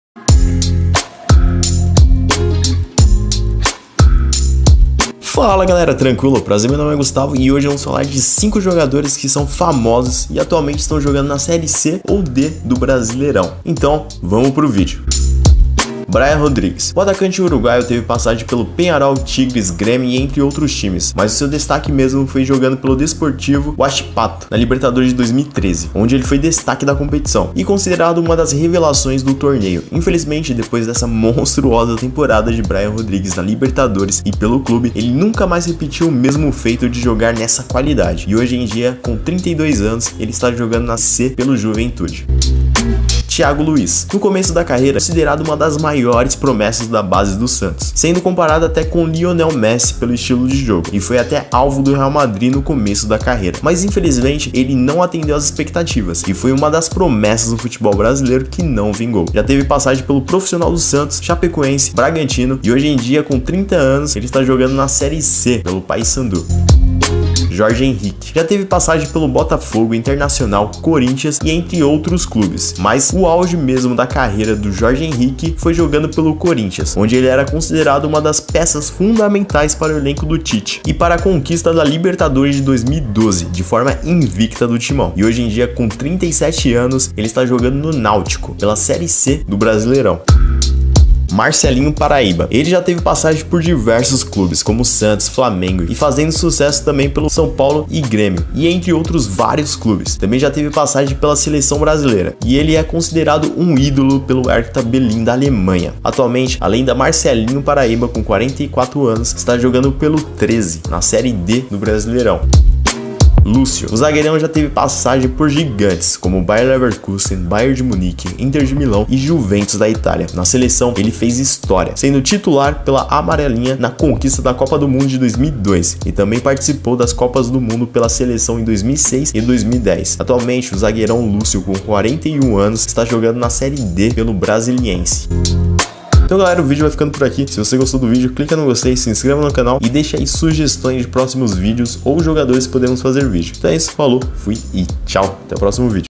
JOGADORES FAMOSOS que ESTÃO JOGANDO a SERIE C e D
Separamos nessa lista 5 jogadores famosos que hoje estão jogando a série C ou D do Brasileirão 2019. Esses são os 5 ex-craques que hoje estão jogando as divisões inferiores do Brasileirão, estão jogando o Campeonato Brasileiro da série C ou D. Nessa lista temos nomes dos craques como: O ex zagueiro da seleção Lúcio,  Braian Rodríguez ex Grêmio, Tiago Luís ex Santos e muito comparado com Messi, Jorge Henrique ex-Corinthians e ex-Internacional, e Marcelinho Paraíba.